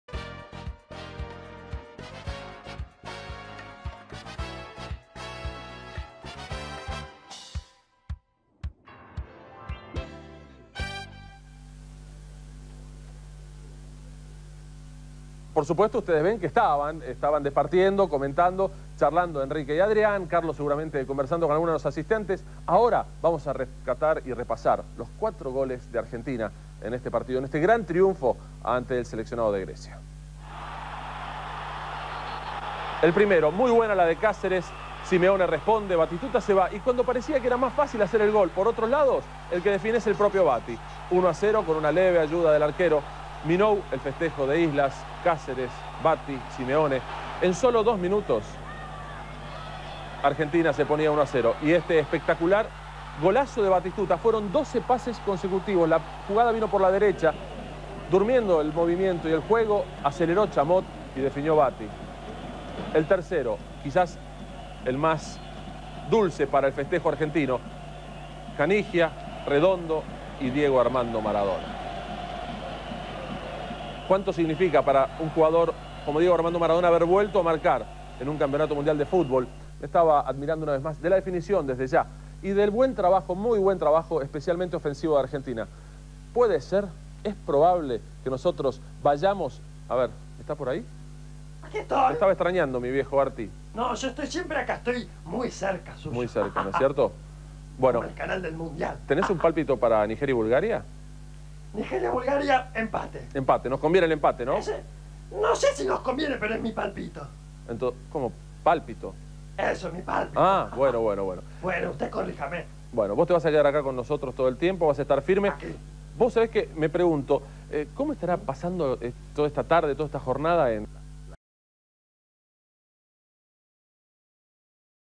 Repaso de goles del 21 de junio 1994 (incompleto)
donátusvhs Lo que se pudo salvar. Rememorando lo que ocurrió en un día como pasado-mañana pero de hace 29 años con el socito CGI que llamaron "Arti" en honor a Artear.